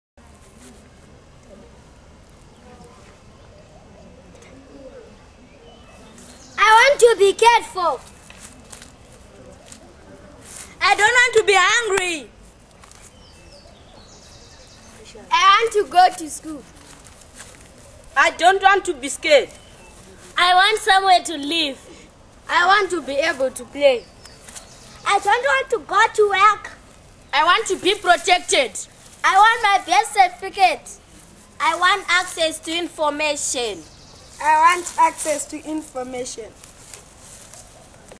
Children's Rights
A group of children discussing their rights